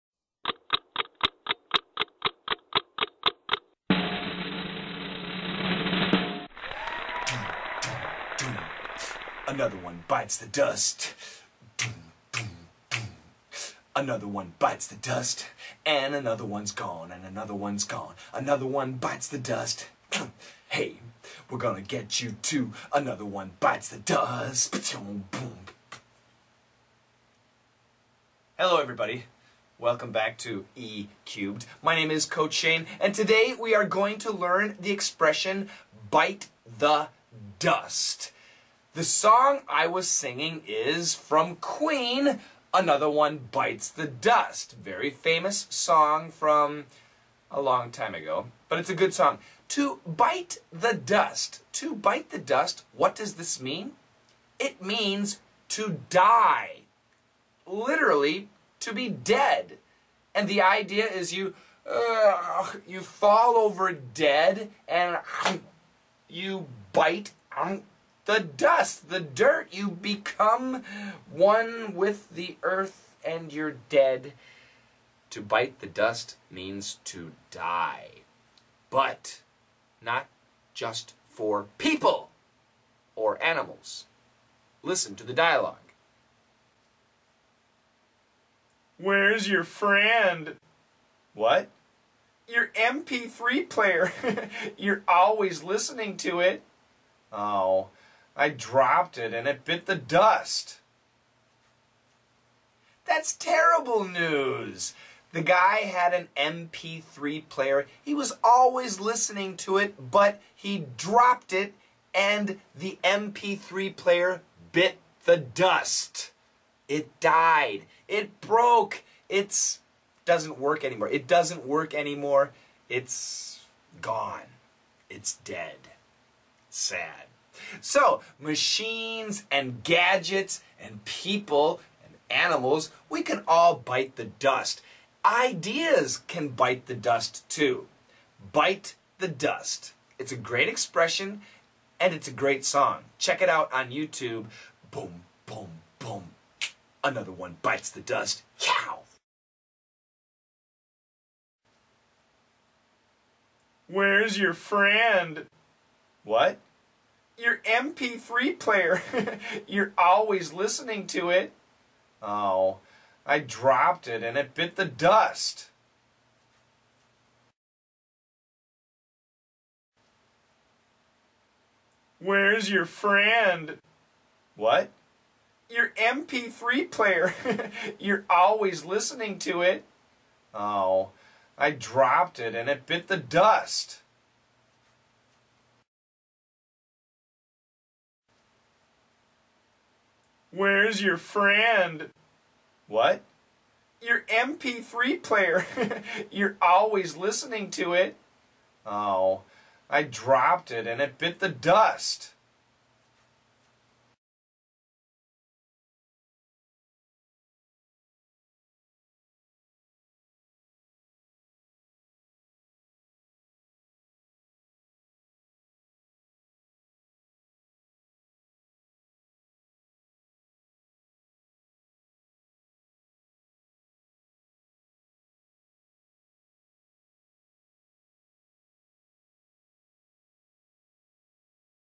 Learn English: Daily Easy English Expression 0113 -- 3 Minute English Lesson: (to) bite the dust
Today's E-cubed: Bite the dust

Where's your friend?
Who?
Your MP3 player! You're always listening to it.
I dropped it and it bit the dust~

This is a video for ESL (English as a Second Language) learners. This video explains a common expression in American English. It also explains typical American pronunciation.